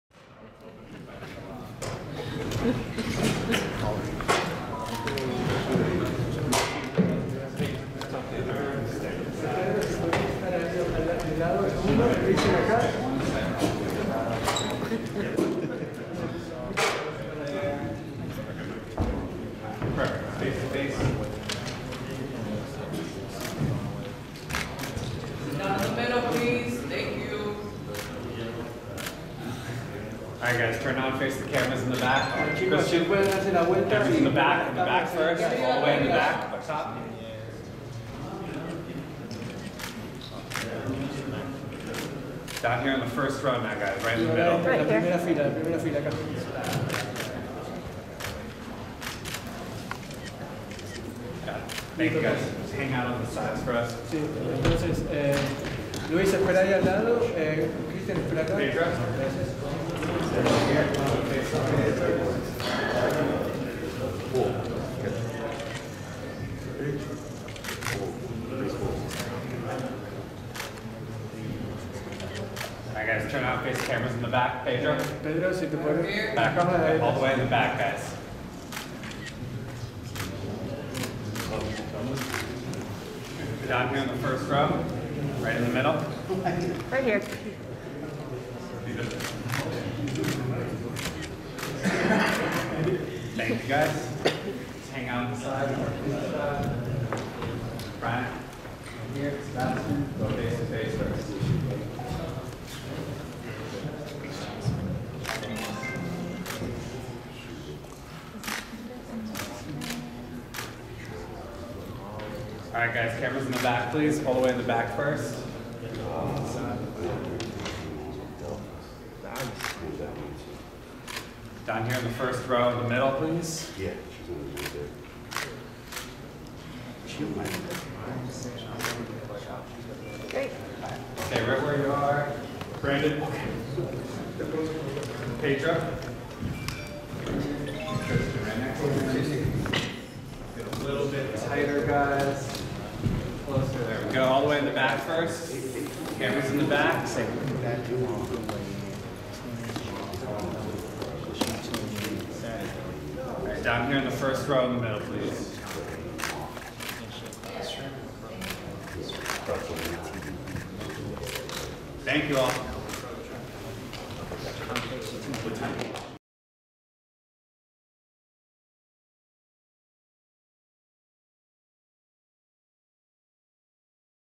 INSANE HEIGHT DIFFERENCE! SEBASTIAN FUNDORA TOWERS OVER BRIAN MENDOZA & STARES HIM DOWN AT FACE OFF
It's fight week! Top jr. middleweight Sebastian Fundora and contender Brian Mendoza are moments away from their upcoming April 8 showdown at Dignity Health Sports Park in Carson, California. You don't want to miss what goes down before, during, and after the fight. Check it out!